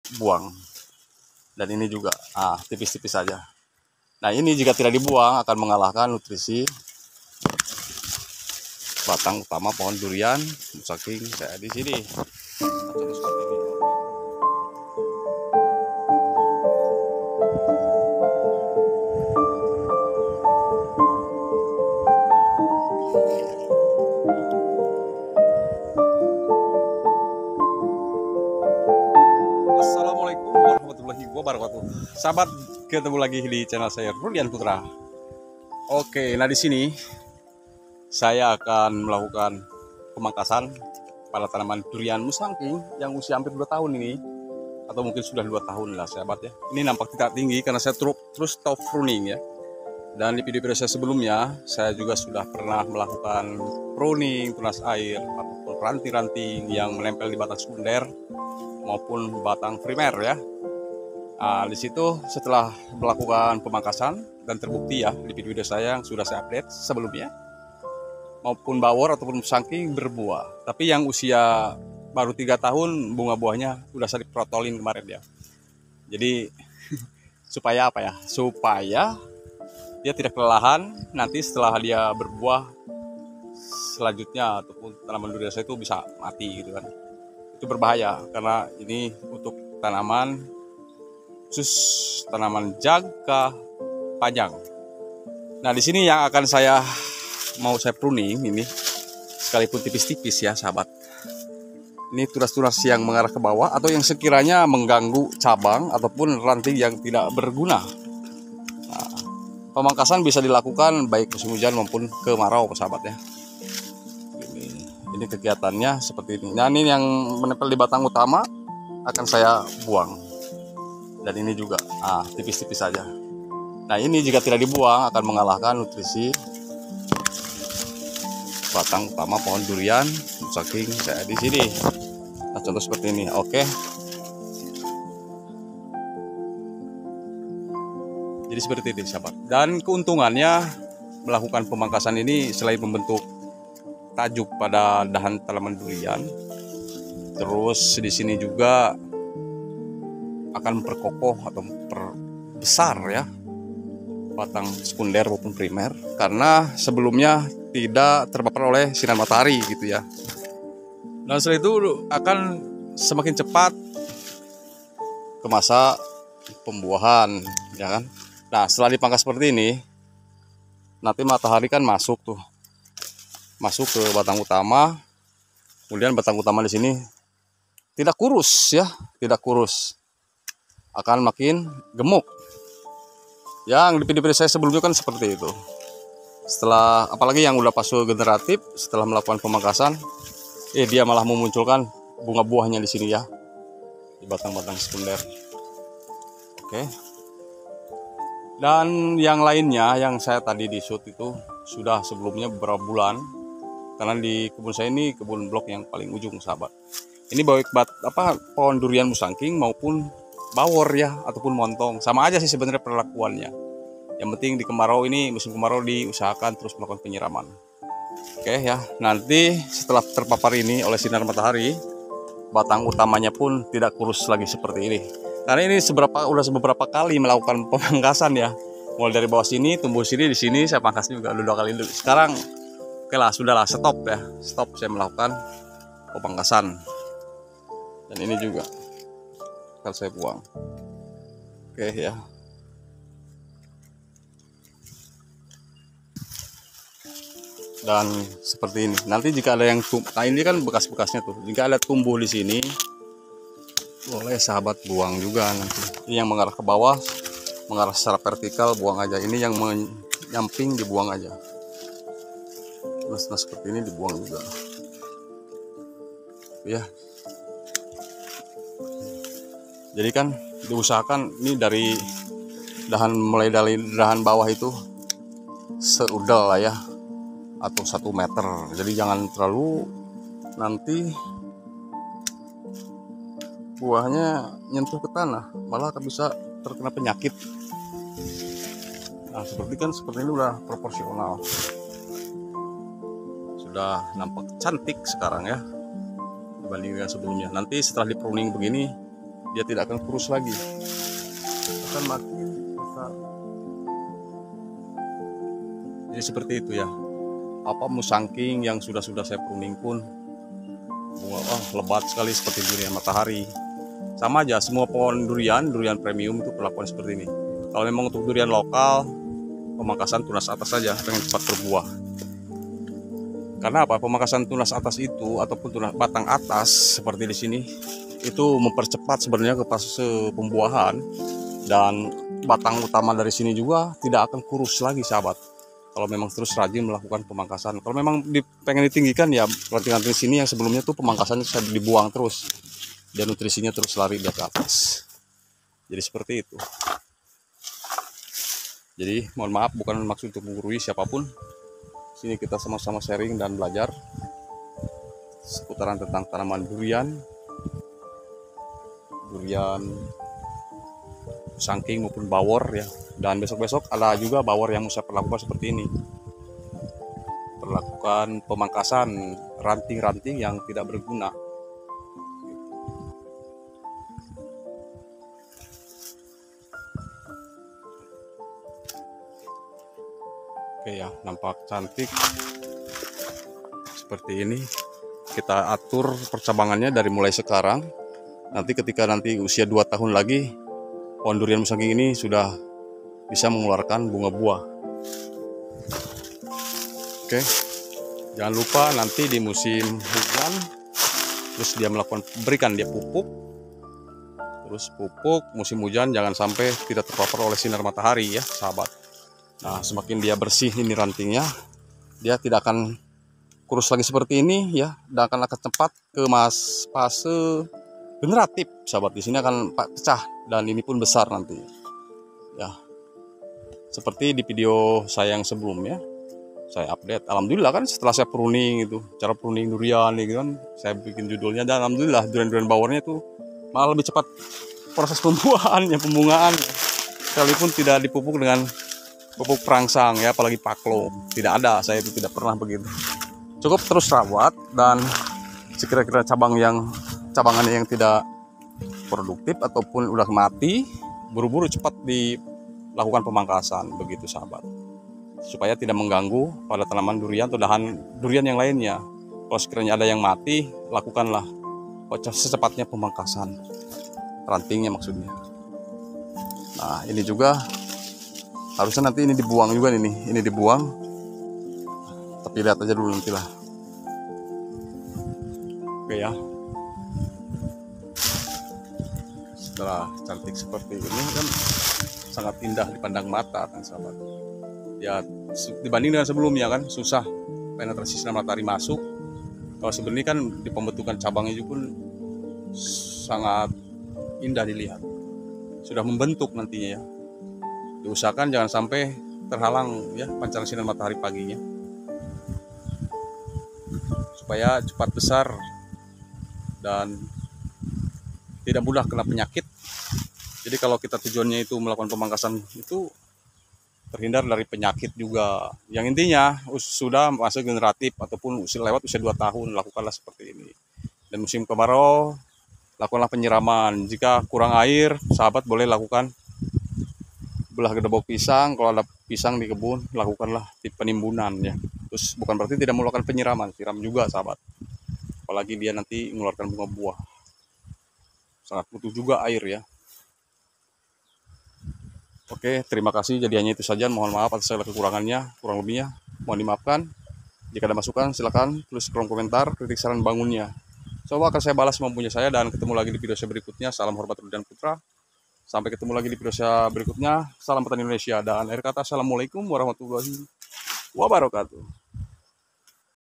Pemangkasan Durian Musangking Agar Cepat Besar Dan Berbuah
pemangkasan saya lakukan pada usia 2 tahun setelah tanam..dan saya lakukan pada saat musim kemarau..silahkan simak video nya sampai selesai..terimakasih